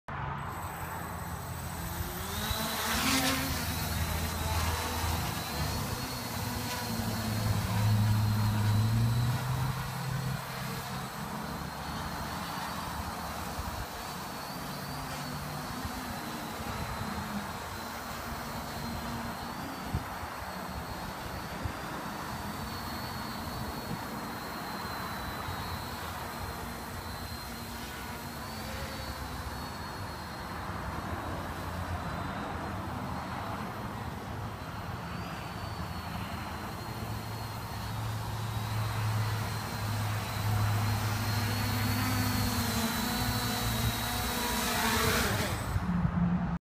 Ghostcopter
I turned my quadcopter into a Ghostcopter for Halloween! Pretty easy with some cheese cloth, foam ball, and LED's.